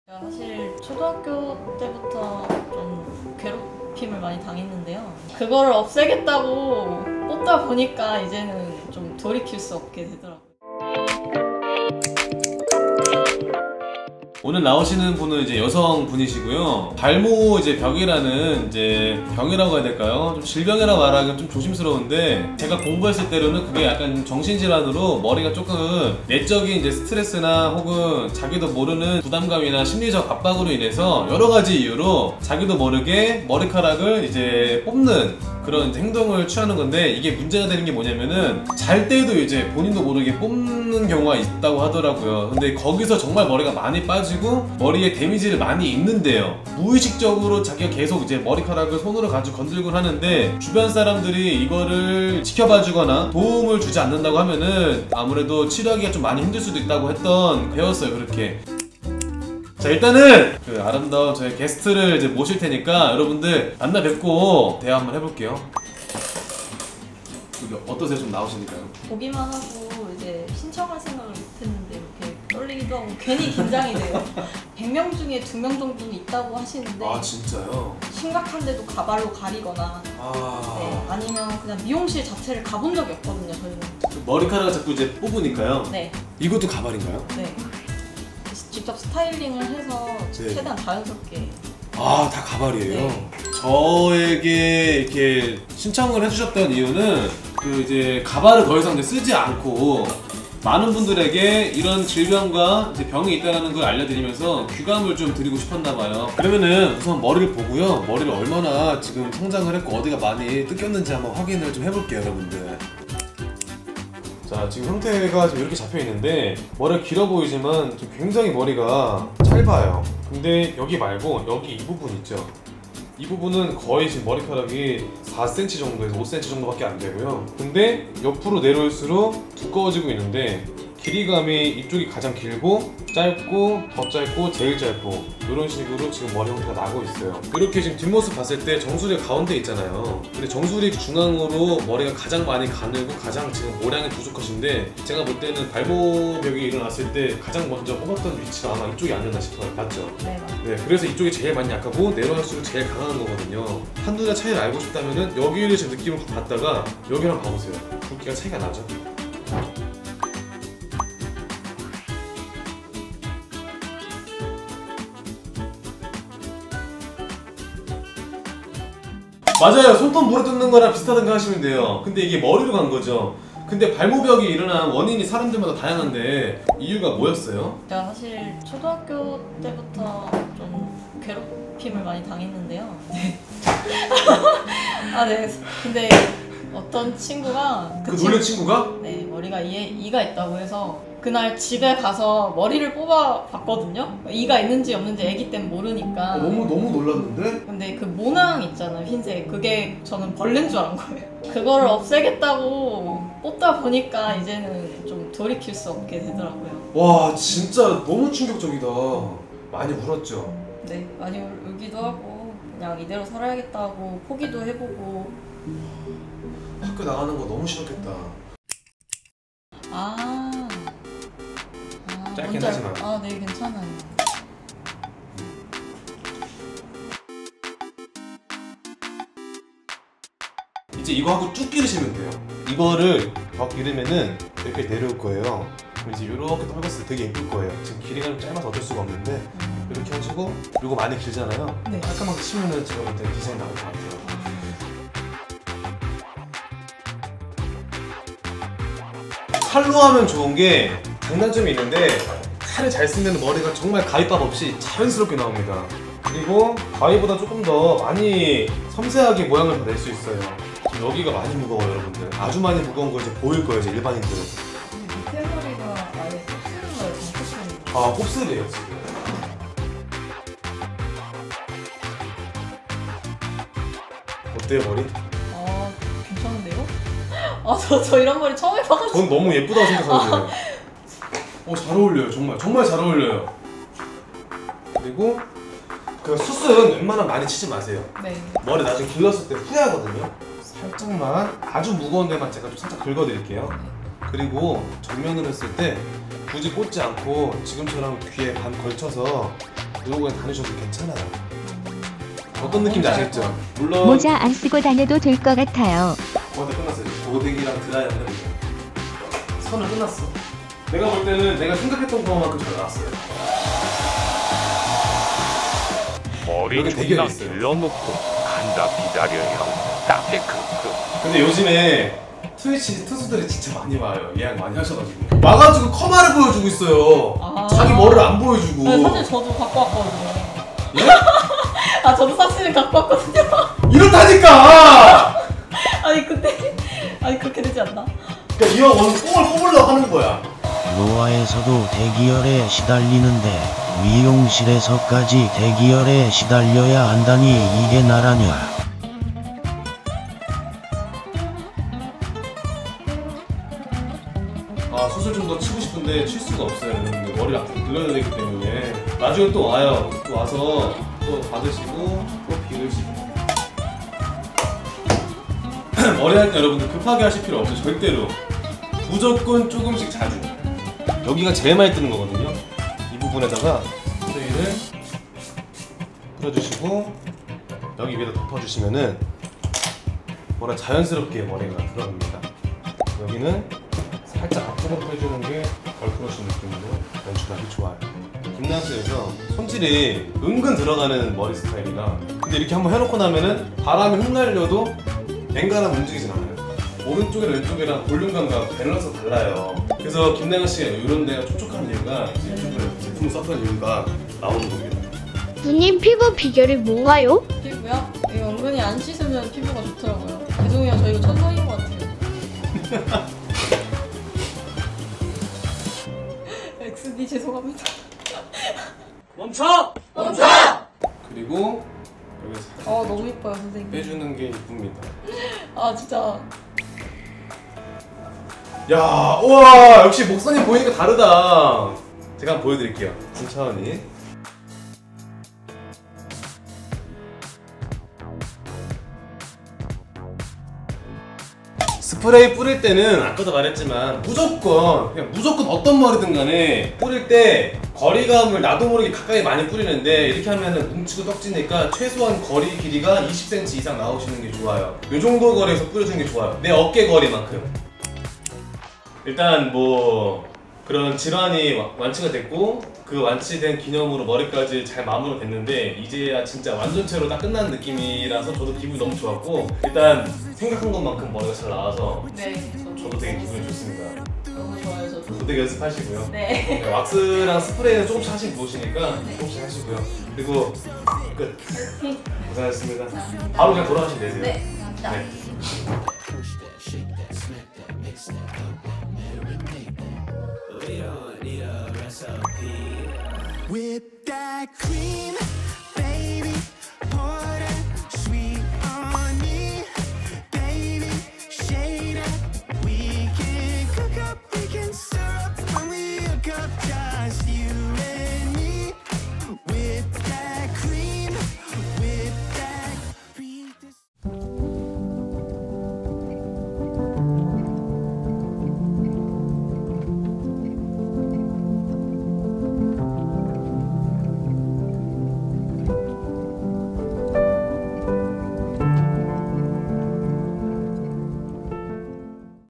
15년동안 괴롭힘으로 자기 머리를 다 뜯어버린 여자의 사연은?
촬영장비: Logitech Stream Cam, UFO PRO
영상편집: 금강
썸네일편집: 금강다뷘

게스트 참여 비용은 무료입니다:)

00:00 인트로
00:14 발모벽이란?
00:37 발모벽이 무서운 이유
01:00 게스트분 등장
01:25 게스트분 가발사진
01:50 게스트분 머리상태 분석
02:39 편집자 센스
02:52 발모벽에 걸린 이유
03:47 딱 맞는 머리는? 커트시작
05:56 윤곽 1
06:19 숏컷 팁
07:01 윤곽 2
08:06 결과
08:36 감동의 메세지